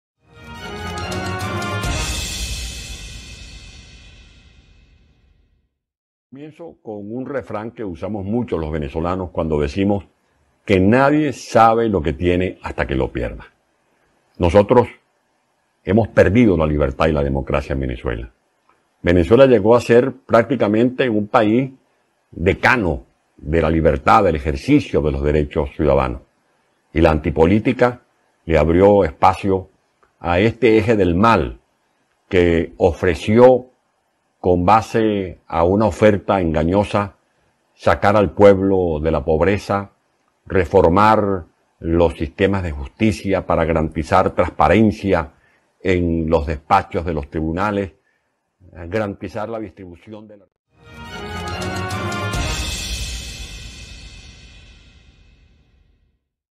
MENSAJE ANTONIO LEDEZMA A LOS CIUDADANOS ARGENTINOS
La Democracia es el único sistema de gobierno que permite a los ciudadanos vivir en paz y en libertad. Mensaje a los ciudadanos Argentinos de Antonio Ledezma